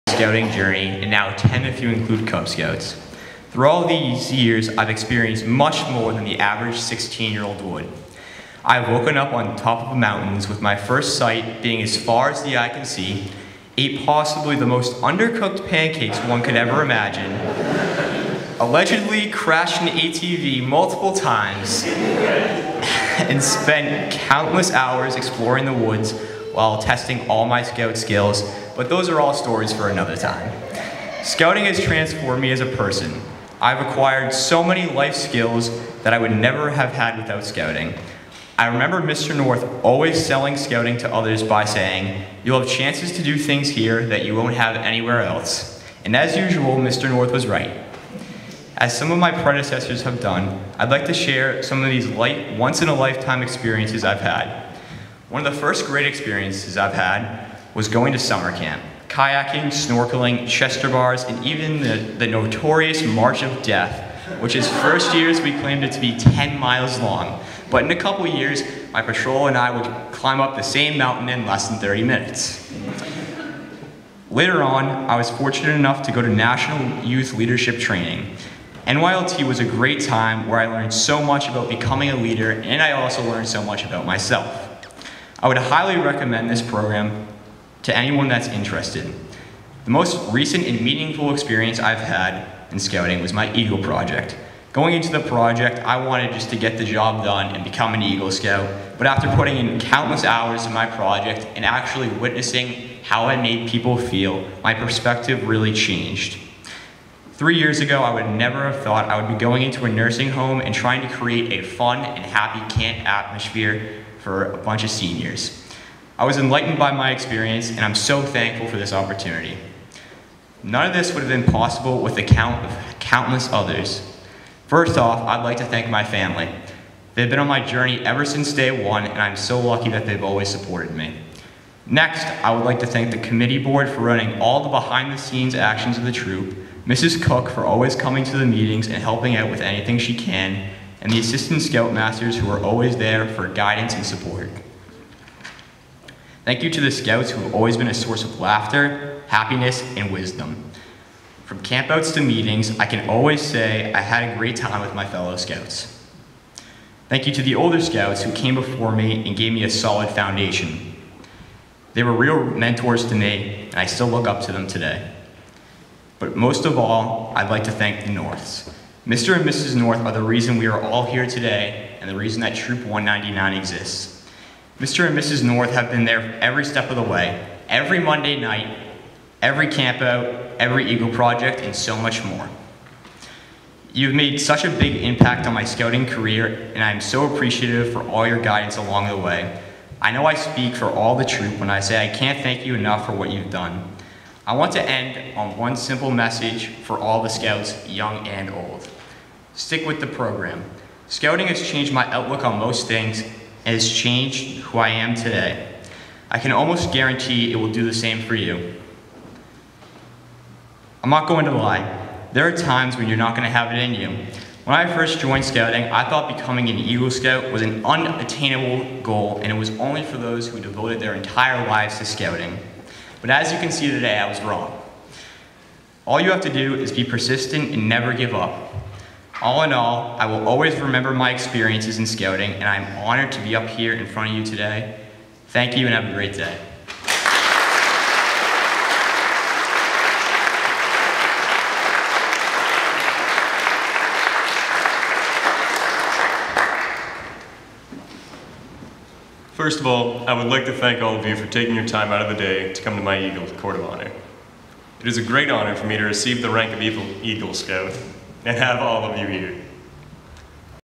William Tuzik Eagle 3
William Tuzik speech on receiving the honor of Eagle Scout 5/26/18